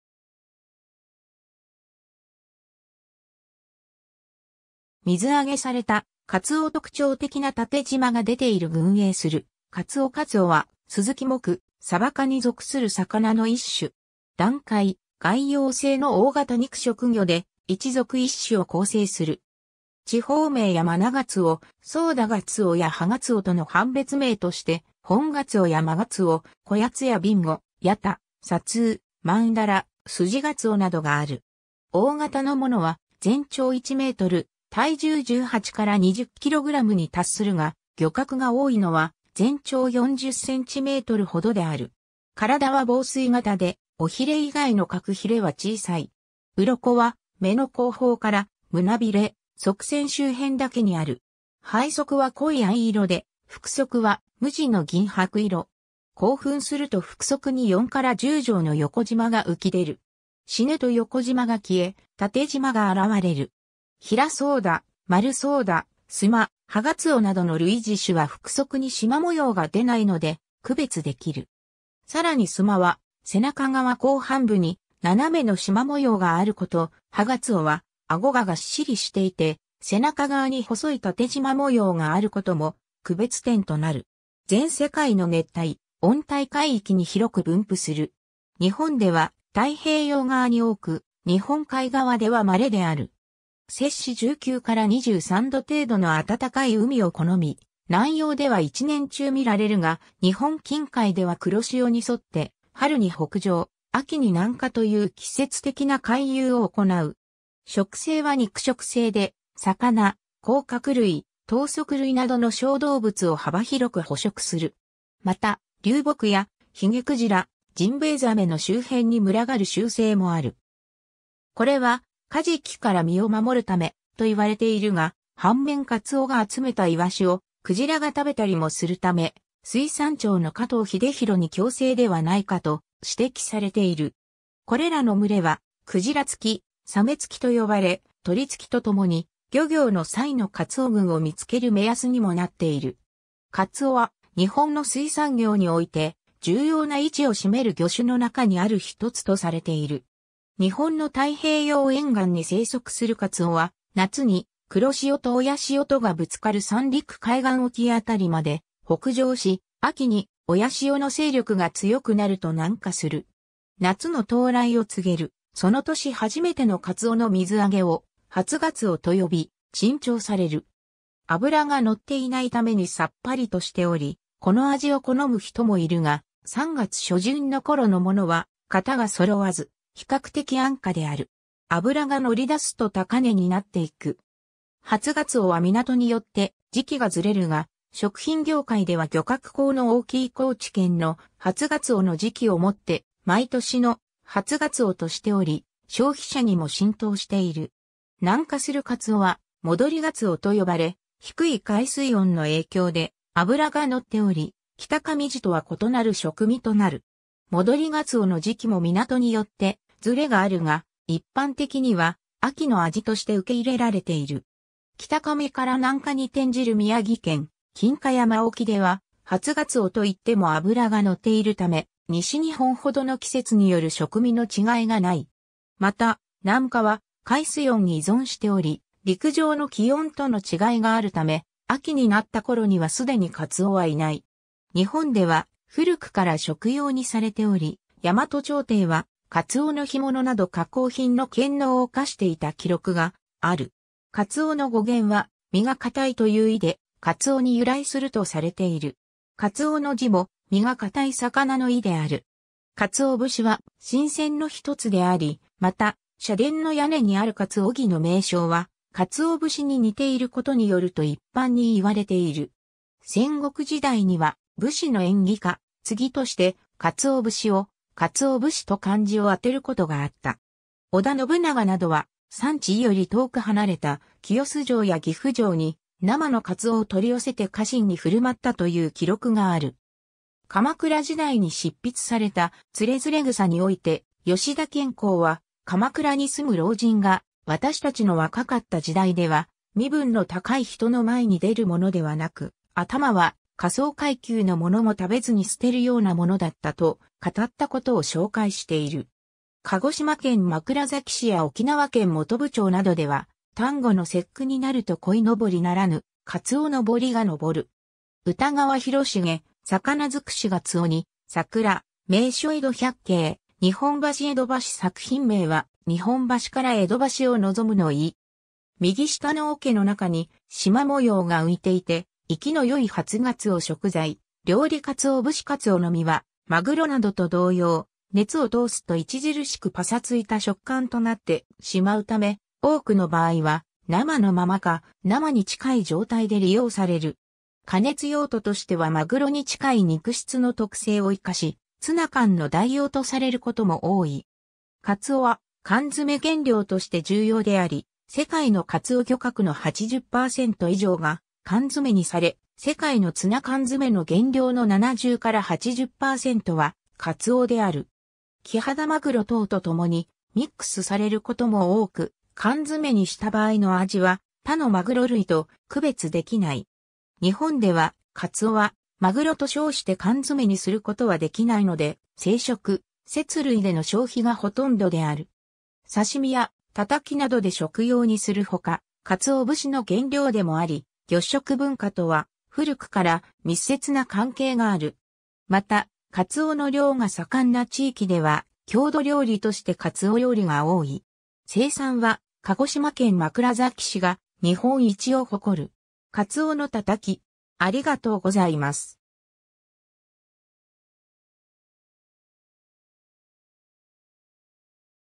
カツオ
水揚げされたカツオ 特徴的な縦縞が出ている 群泳するカツオ カツオ（鰹、松魚、堅魚、英: skipjack tuna、学名 Katsuwonus pelamis ）は、スズキ目・サバ科に属する魚の一種。
暖海・外洋性の大型肉食魚で、1属1種（カツオ属 Katsuwonus）を構成する。
 地方名やマナガツオ、ソウダガツオやハガツオとの判別名として、ホンガツオやマガツオ（各地）、コヤツやビンゴ（仙台 : 若魚）、ヤタ（仙台 : 成魚）、サツウ（小名浜）、マンダラ（北陸）、スジガツオ（和歌山県・高知県）などがある。
 大型のものは全長1m・体重18〜20kgに達するが、漁獲が多いのは全長40cm程である。
体は紡錘形で尾鰭以外の各鰭は小さい。
鱗は目の後方から胸鰭・側線周辺だけにある。
 背側は濃い藍色で、腹側は無地の銀白色。
興奮すると腹側に4-10条の横縞が浮き出る。
死ぬと横縞が消え、縦縞が現れる。
ヒラソウダ、マルソウダ、スマ、ハガツオなどの類似種は腹側に縞模様が出ないので区別できる。
さらにスマは背中側後半部に斜めの縞模様があること、ハガツオは顎ががっしりしていて背中側に細い縦縞模様があることも区別点となる。
 全世界の熱帯・温帯海域に広く分布する。
日本では太平洋側に多く、日本海側では稀である。
摂氏19 - 23度程度の暖かい海を好み、南洋では一年中見られるが、日本近海では黒潮に沿って春に北上、秋に南下という季節的な回遊を行う。
食性は肉食性で、魚、甲殻類、頭足類などの小動物を幅広く捕食する。
 また、流木やヒゲクジラ（主にニタリクジラ、カツオクジラ）、ジンベエザメの周辺に群がる習性もある。
これはカジキから身を護るため{と言われているが、反面カツオが集めた鰯を鯨が食べたりもするため、水産庁の加藤秀弘に共生ではないかと指摘されている。
これらの群れは「鯨付き」「鮫付き」と呼ばれ、「鳥付き」とともに漁業の際のカツオ群を見つける目安にもなっている。
 カツオは、日本の水産業において重要な位置を占める魚種の中にある1つとされている。
 日本の太平洋沿岸に生息するカツオは、夏に黒潮と親潮とがぶつかる三陸海岸沖辺りまで北上し、秋に親潮の勢力が強くなると南下する。
夏の到来を告げるその年初めてのカツオの水揚げを「初鰹」（はつがつお）と呼び、珍重される。
脂が乗っていないためにさっぱりとしており、この味を好む人もいるが、3月初旬の頃のものは型が揃わず、比較的安価である。
脂が乗り出すと高値になっていく。
 初鰹は港によって時期がずれるが、食品業界では漁獲高の大きい高知県の初鰹の時期（4月 - 6月頃）をもって毎年の「初鰹」としており、消費者にも浸透している。
南下するカツオは「戻り鰹」と呼ばれ、低い海水温の影響で脂が乗っており、北上時とは異なる食味となる。
戻り鰹の時期も港によってずれがあるが、一般的には秋の味として受け入れられている。
 北上から南下に転じる宮城県・金華山沖では、「初鰹」と言っても脂が乗っているため、西日本ほどの季節による食味の違いがない。
また、南下は海水温に依存しており、陸上の気温との違いがあるため、秋になった頃には既にカツオはいない。
 日本では古くから食用にされており、大和朝廷は鰹の干物（堅魚）など加工品の献納を課していた記録がある。
カツオの語源は「身が堅い」という意で堅魚（かたうお）に由来するとされている。
「鰹」の字も「身が堅い魚」の意である。
 鰹節（干鰹）は神饌の一つであり、また、社殿の屋根にある鰹木の名称は、鰹節に似ていることによると一般に云われている。
戦国時代には武士の縁起かつぎとして、鰹節を「勝男武士」と漢字をあてることがあった。
織田信長などは、産地より遠く離れた清洲城や岐阜城に生の鰹を取り寄せて家臣に振る舞ったという記録がある。
 鎌倉時代に執筆された『徒然草』において、吉田兼好は鎌倉に住む老人が「わたしたちの若かった時代では身分の高い人の前に出るものではなく、頭は下層階級の者も食べずに捨てるような物だった」と語った事を紹介している（『徒然草』第119段）。
 鹿児島県枕崎市や沖縄県本部町などでは、端午の節句になるとこいのぼりならぬ「カツオのぼり」が上る。
 歌川広重『魚づくし 鰹に桜』 『名所江戸百景』「日本橋江戸ばし」　作品名は日本橋から江戸橋を望むの意。
右下の桶の中に、縞模様が浮いていて活きのよい初鰹 食材・料理 鰹節 カツオの身はマグ...